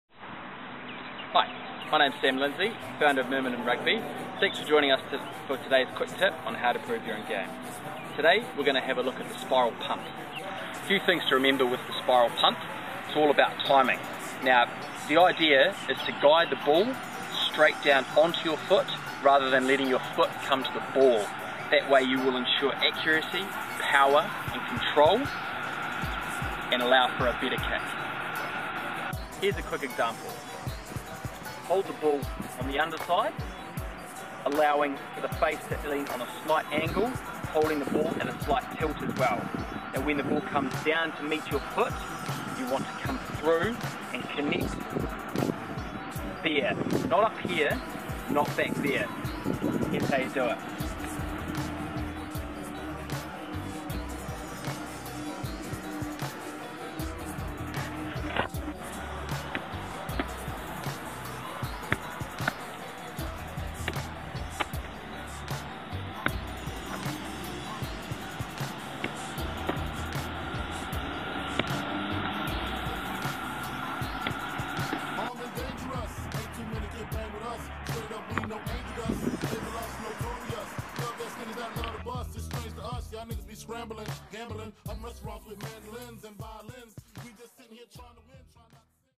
Rugby Tutorial - Spiral Punt - Myrmidon Rugby
Here is a short tutorial video on how to properly execute a torpedo, or spiral punt, in rugby. Included are your hand positioning, timing, power generation, accuracy and running examples.